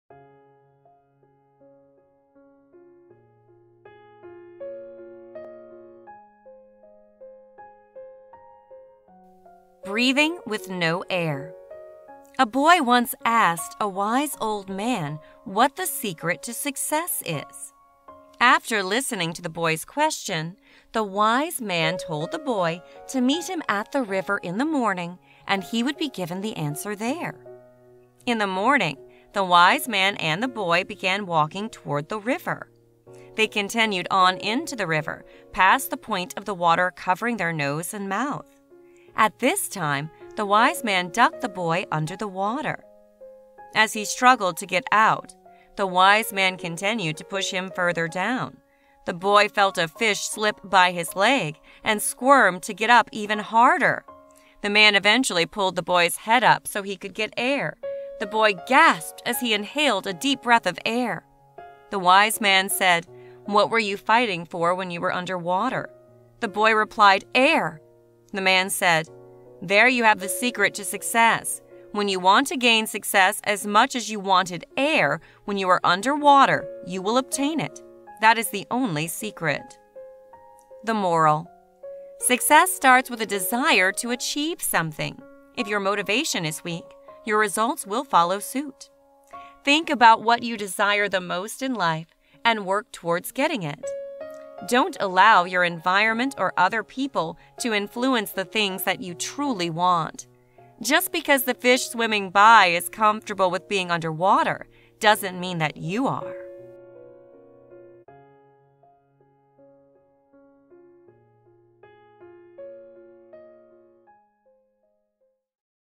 Breathing with no air - A lesson from a wise old man
A boy once asked a wise old man what the secret to success is. After listening to the boy’s question, the wise man told the boy to meet him at the river in the morning and he would be given the answer there.